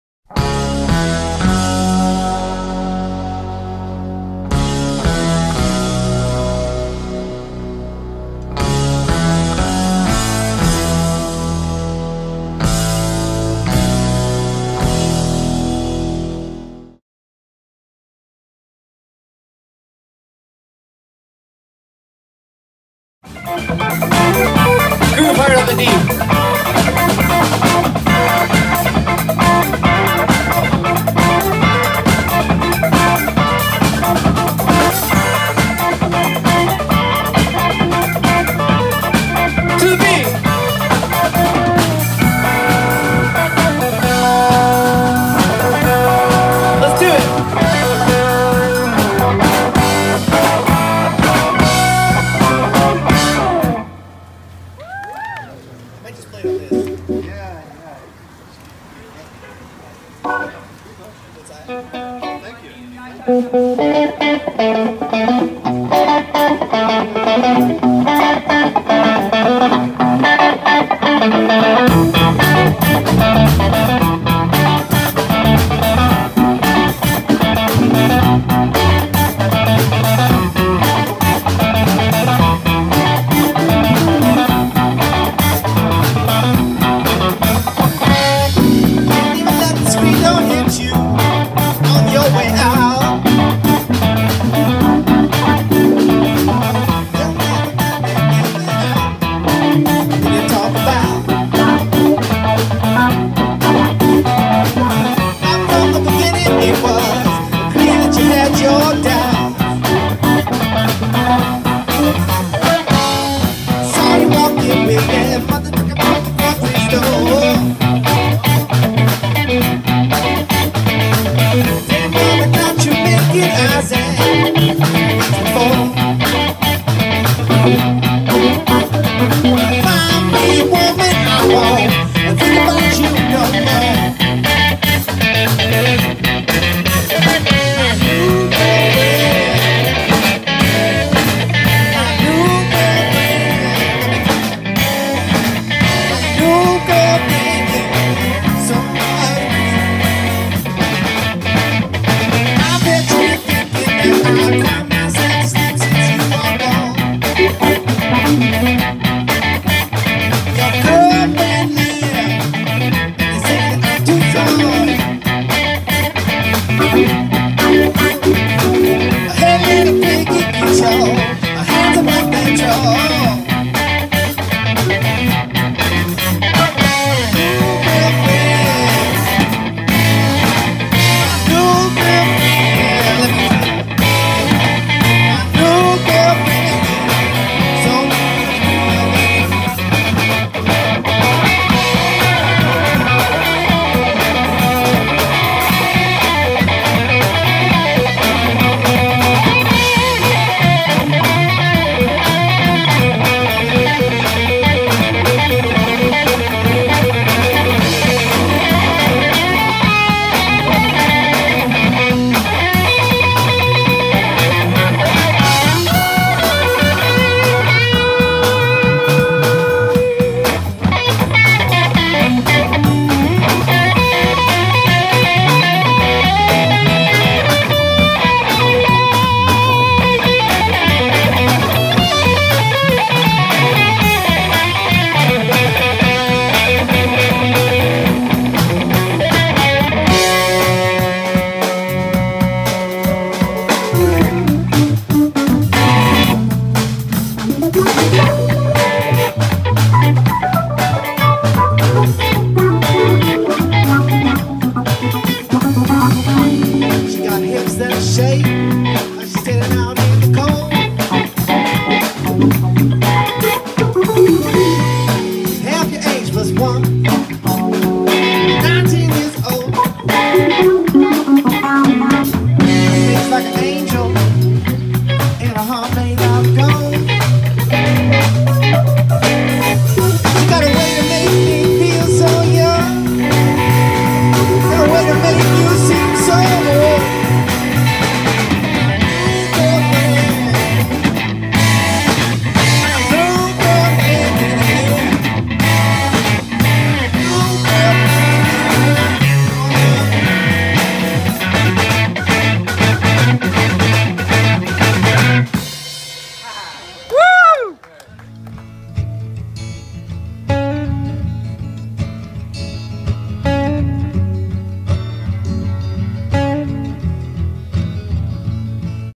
Extra Terrestrial Rhinoceros Thronbury Parkette
Released Ahead of Schedule ! By popular demand.!!! Extra Terrestrial Rhinoceros, aka ET Rhino is a 2-piece band of musicians that were born on the wrong planet - dedicated to making you bust a move!
Jayden Grahlman - Guitar/Vocals
Jay Stiles - Rhodes/Organ/Vocals
You can check them out and or contact them on Facebook.
 E T Rhino
I hope you enjoy this . More E T Rhino  to come in a few days .
Love and Peace from Canada  We are Youtube